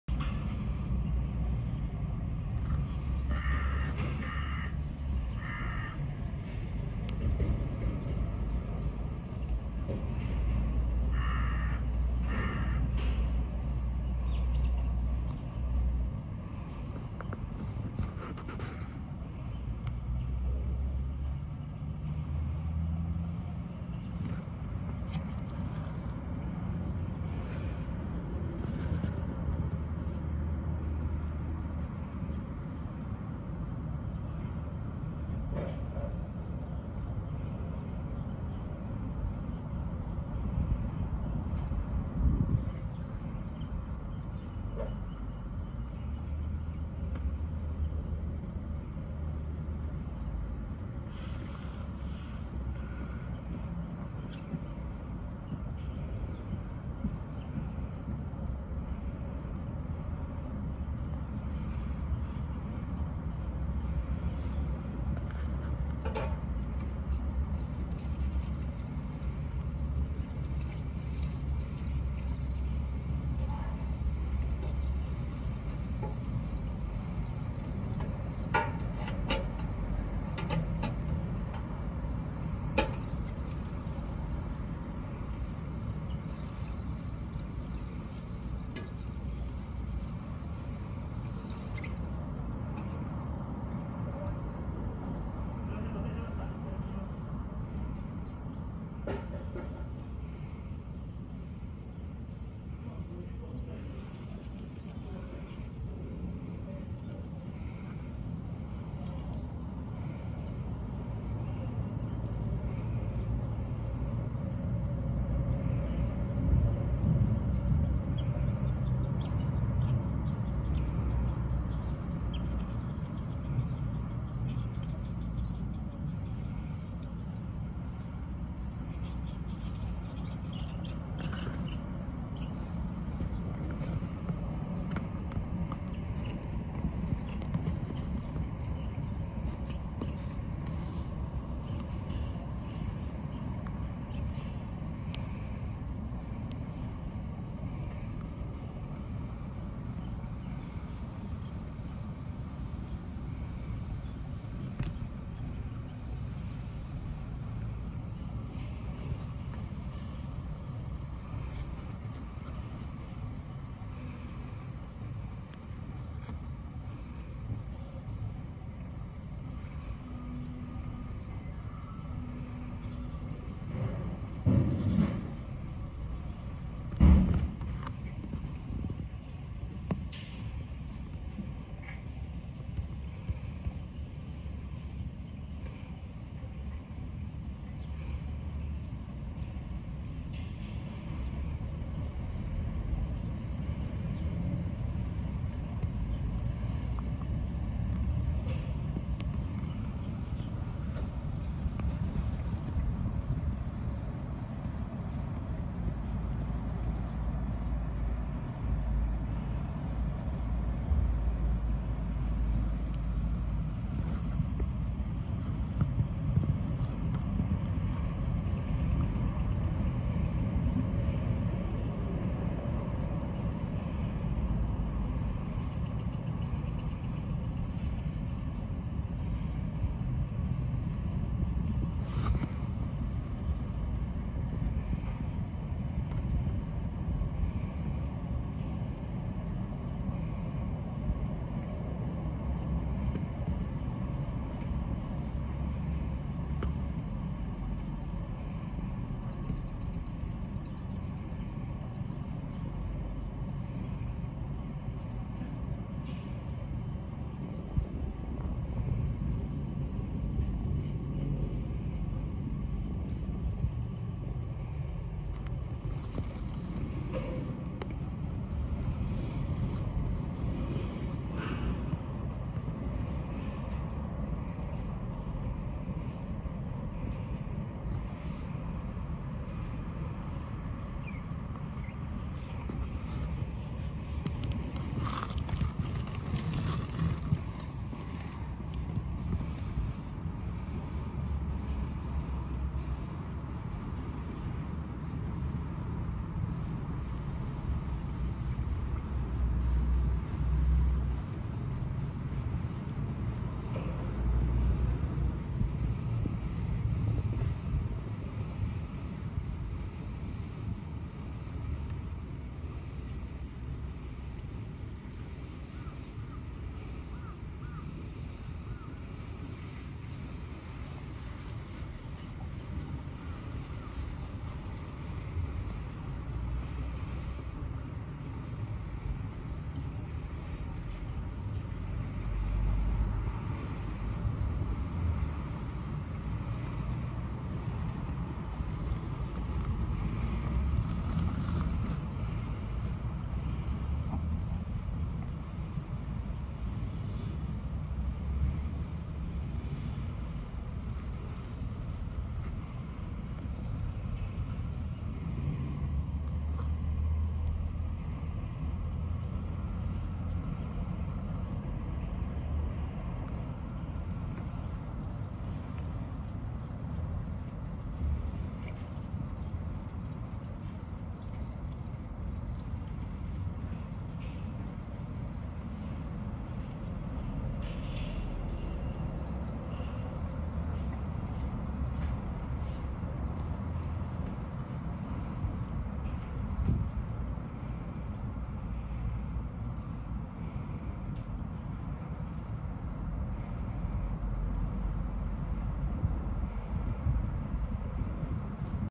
2022.2アライグマ晩ご飯
展示場から厩舎に帰ったアライグマの晩ご飯。
檻に入っているのは怪我を治療中の子です。